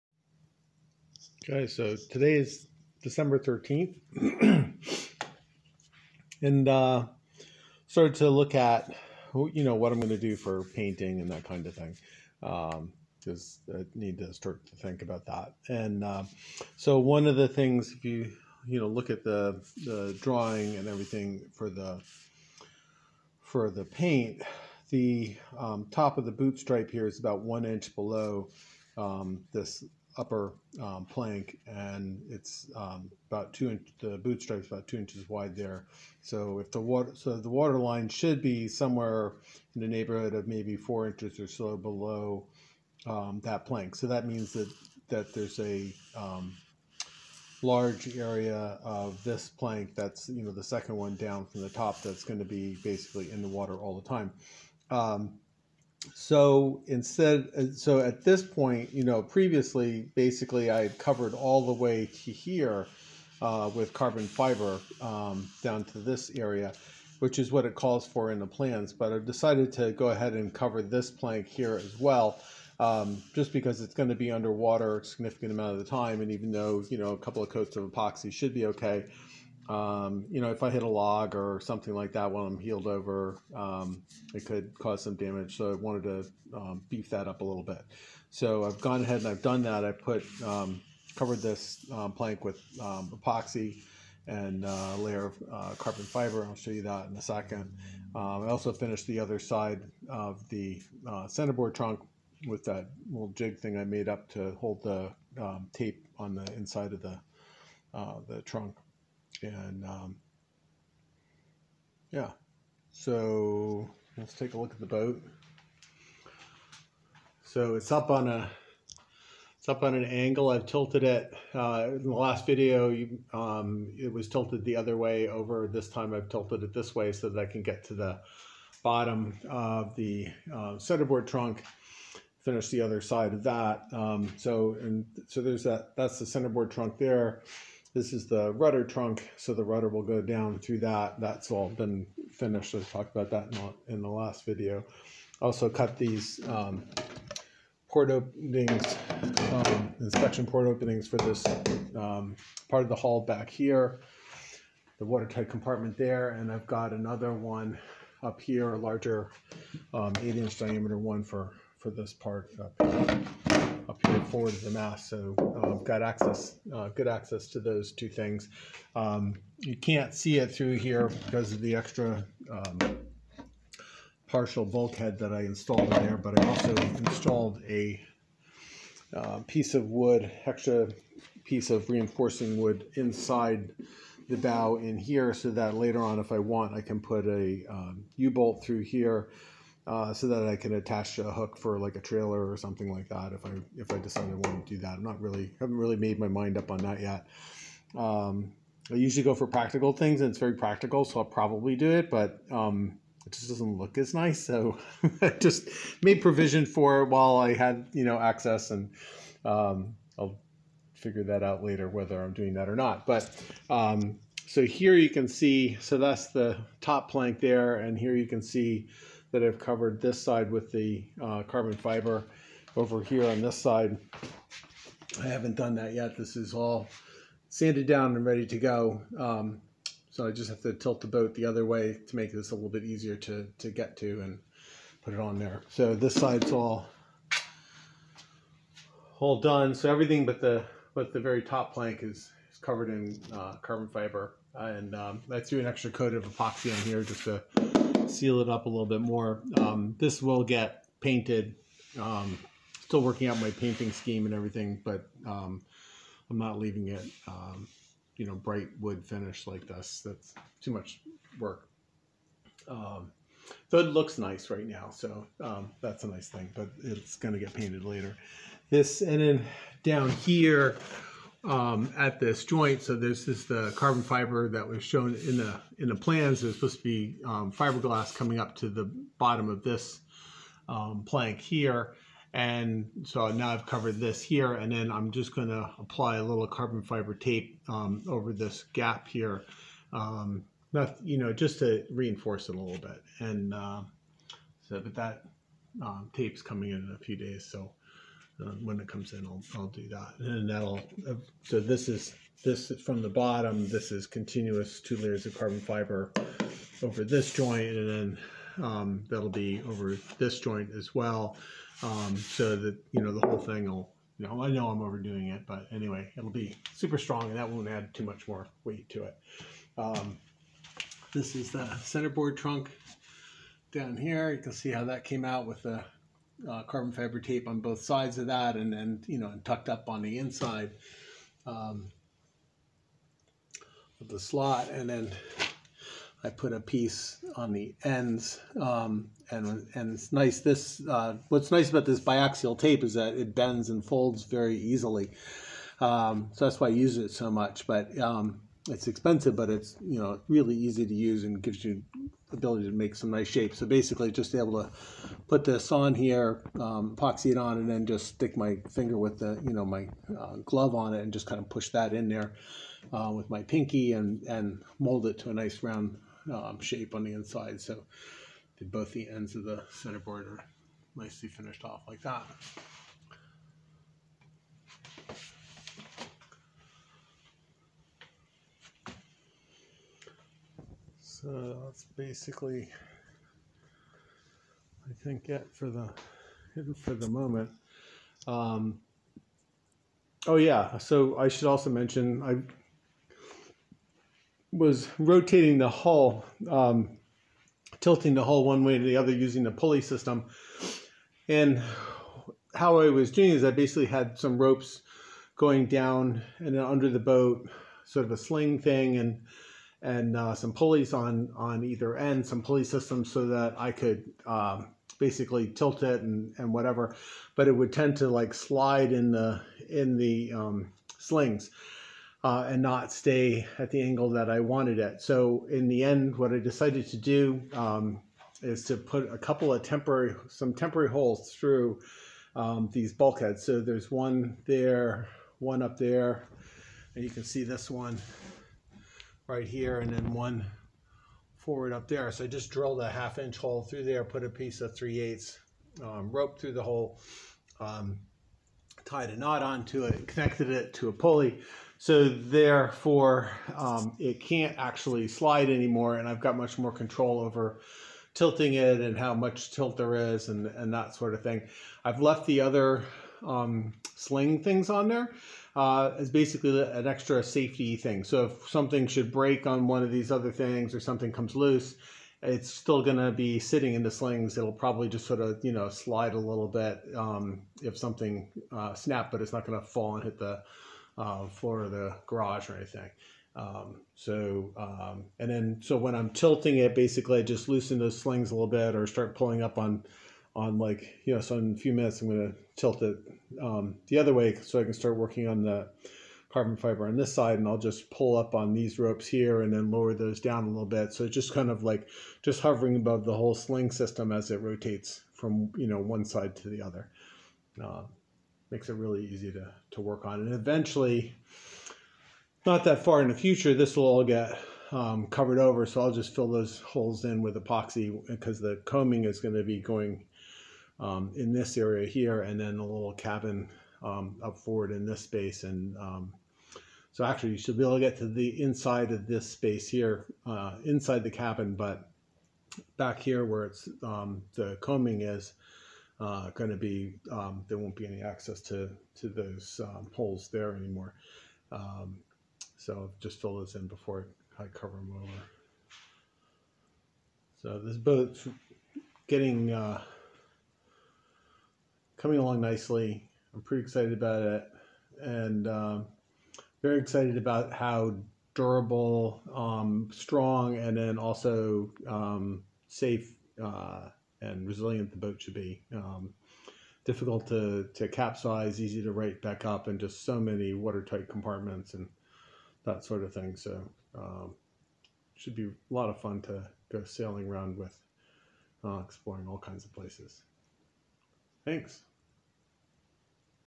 CLC Guider December 13
I talk about painting, additional carbon fiber on two planks, rope, pulley, sling system for rotating the boat.